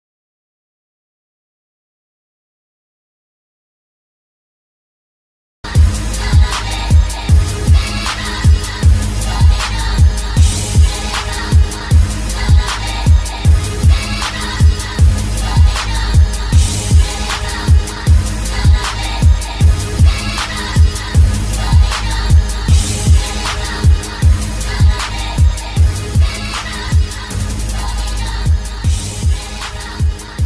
Mario Haas Freistoßtor LASK - Sturm Graz 20.04.2008
Traumhaftes Freistoßtor von Mario Haas in der 79. Minute im Spiel LASK - Sturm Graz am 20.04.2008!

Sturm Graz konnte das damalige Spiel 2:1 gewinnen und sammelte somit wichtige Punkte, im vorletzten Meisterschaftsspiel, für einen Europacup-Startplatz.